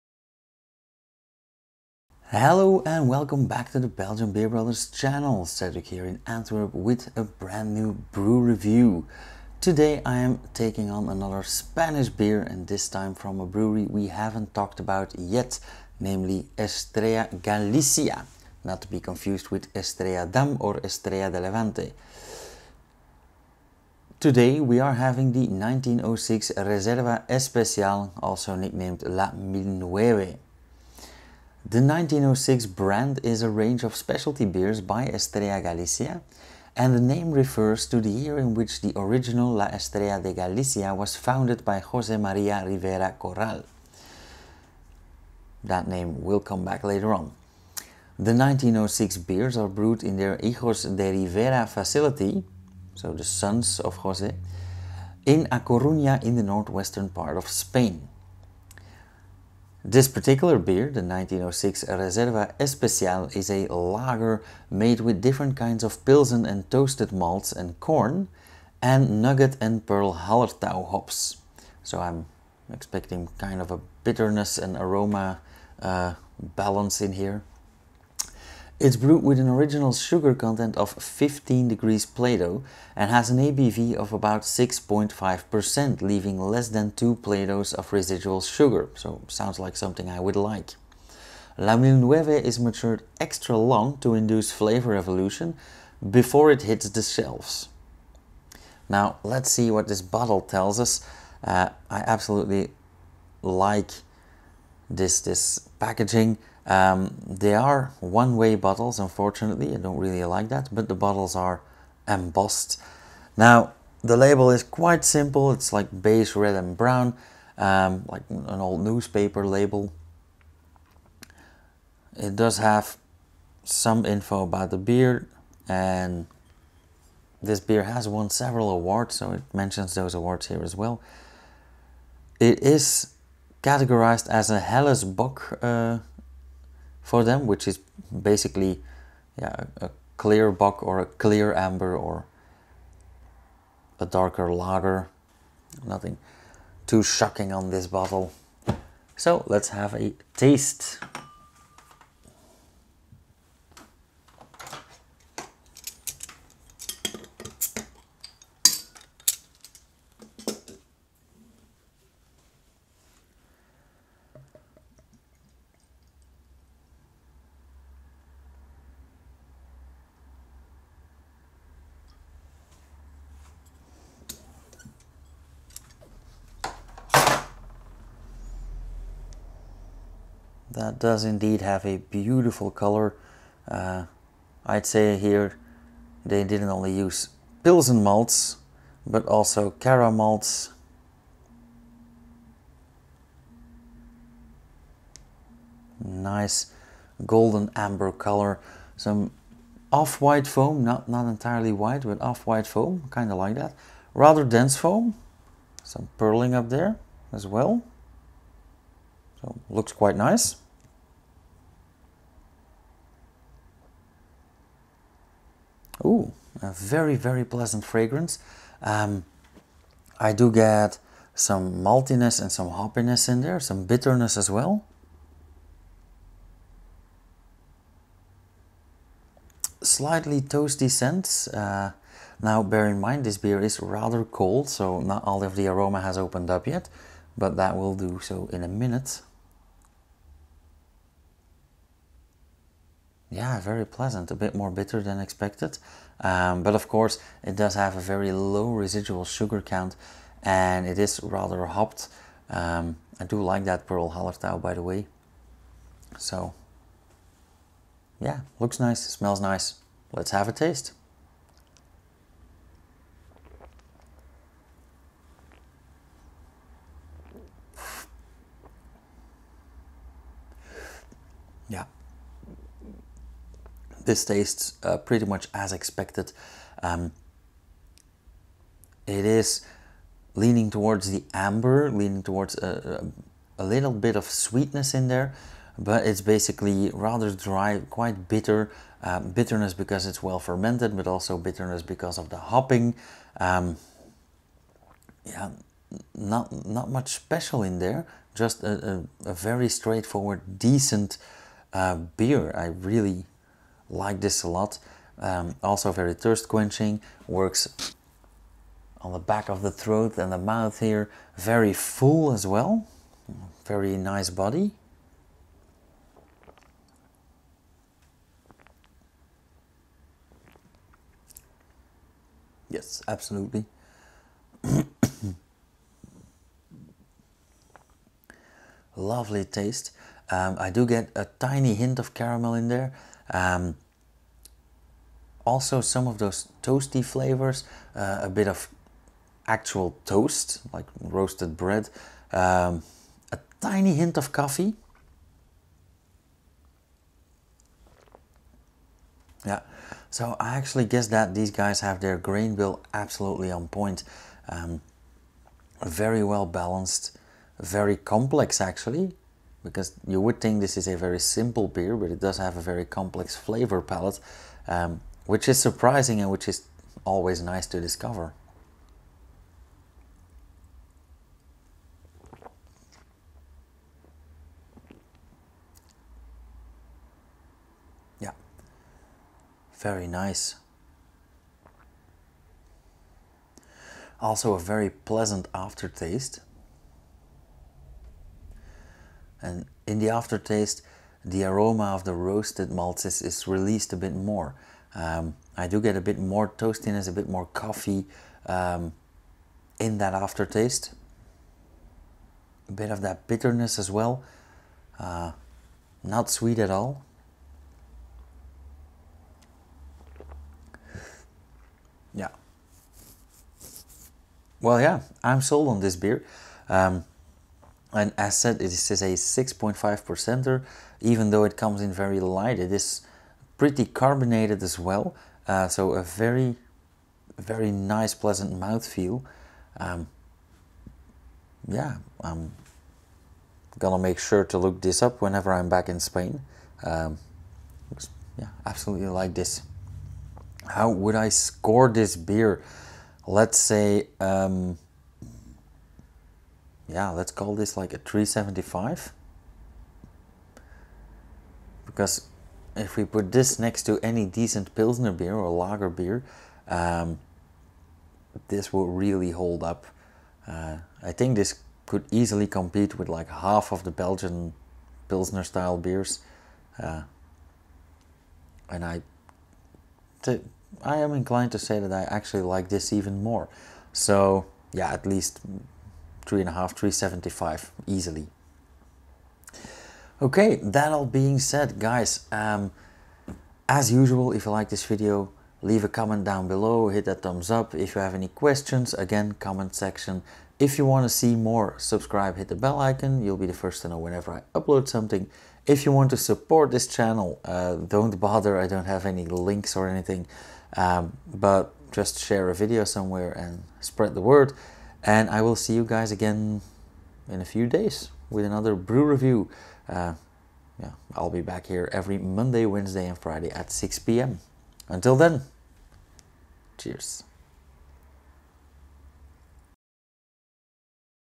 Brew Review 215 - 1906 Reserva Especial (Estrella Galicia)
Let's review another Spanish specialty beer. They call it a Helles Bock and brew it in their century old factory. Let's try it!

00:00 Thumb
00:02 Intro
00:23 About the Beer
01:38 Label & Packaging
02:28 The Pour & Looks
03:31 Fragrance
04:17 Taste & Body
06:23 Aftertaste
08:07 The Verdict